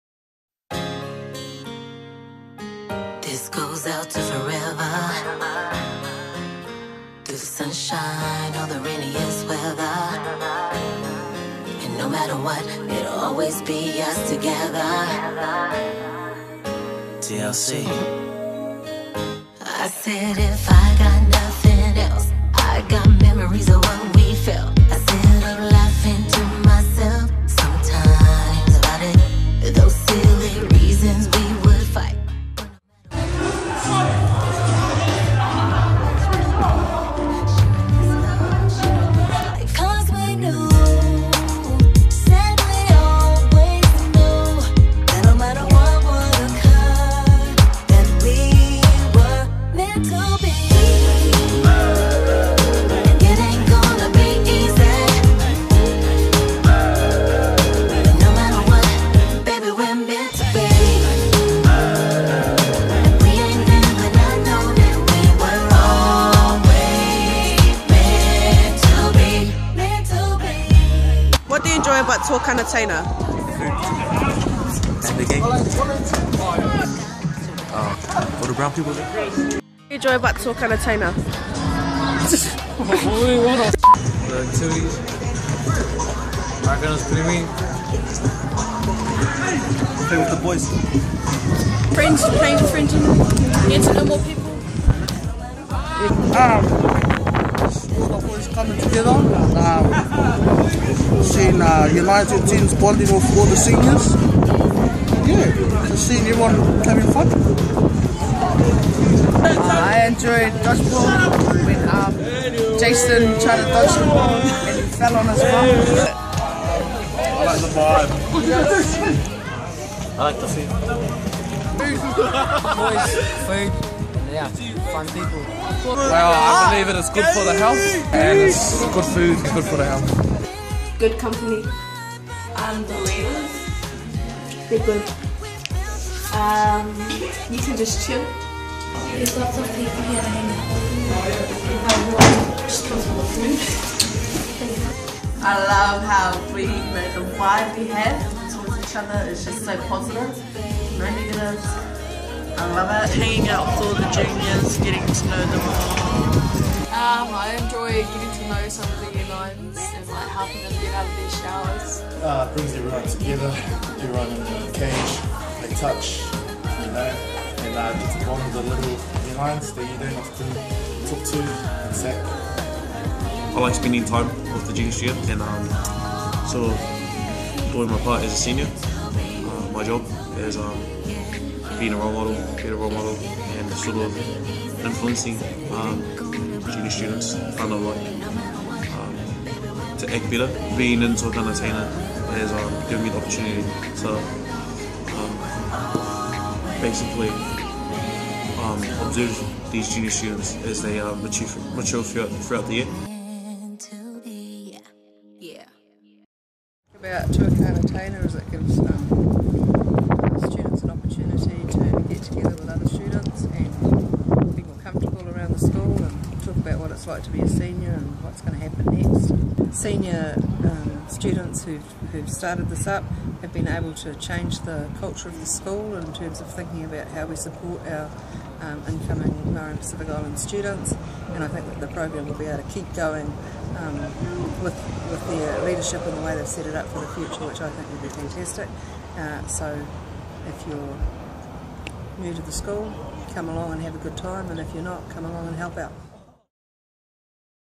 Tuakana Teina
Tuakana Teina is about helping our young brothers and sisters in Tawa College.